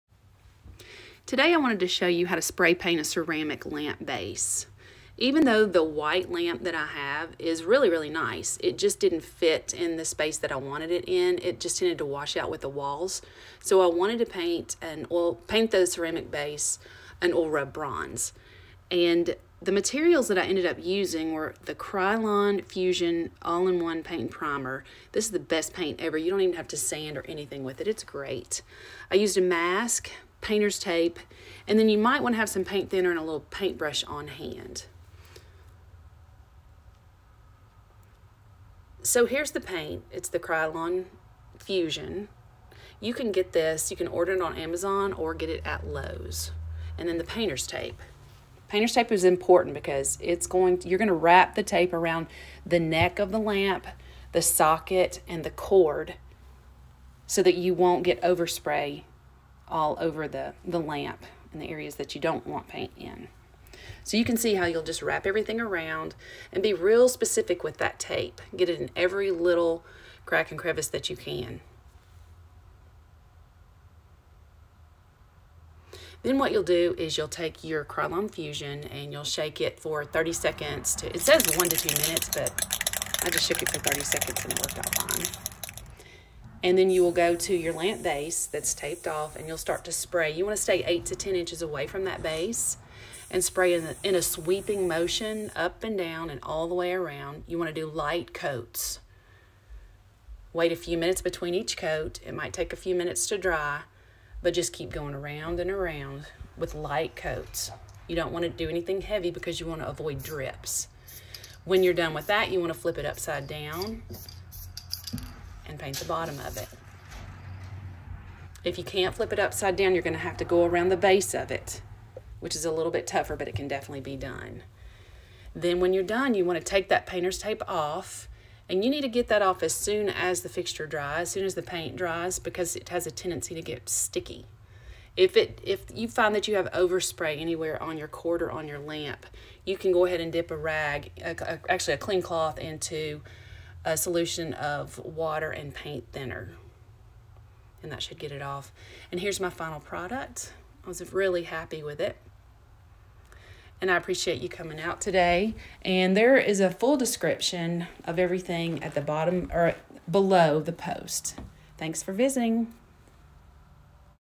How to Spray Paint a Ceramic Lamp Base - Easy DIY Tutorial
An easy way to spray paint a new or old ceramic lamp base.